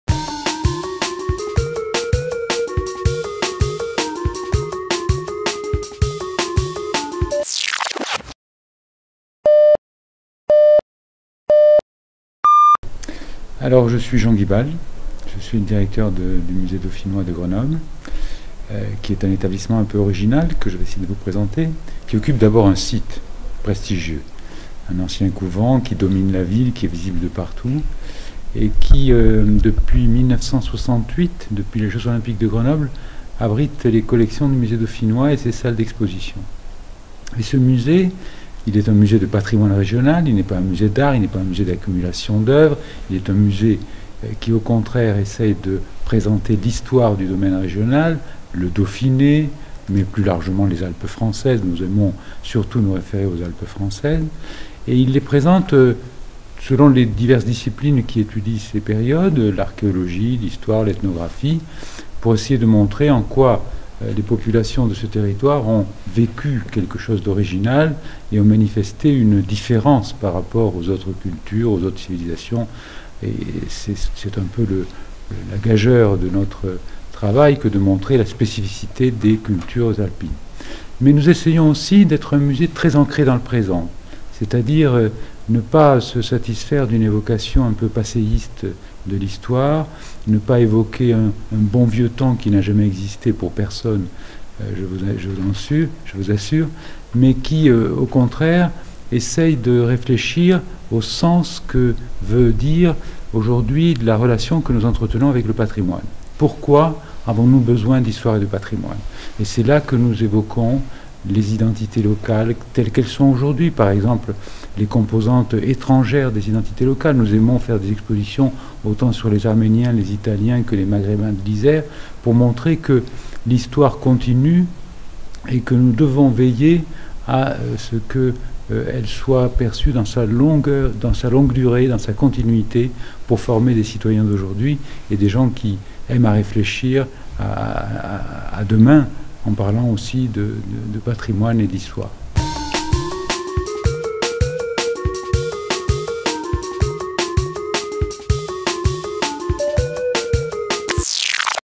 [1/10] Présentation du musée par Jean Guibal - Museomix 2013 au Musée dauphinois de Grenoble
En quelques mots, Jean Guibal nous propose une immersion dans la grande richesse des collections du musée dont il a la direction. Des collections aussi abondantes que variées qui, parce qu'elles ont avant toute chose valeur testimoniale, sont l'occasion pour leur directeur de rappeler que "le patrimoine, ce sont d'abord des gens." Moderne et résolument ancré dans son époque, le Musée dauphinois questionne son temps et pose notamment la question de la relation à l'histoire et au patrimoine.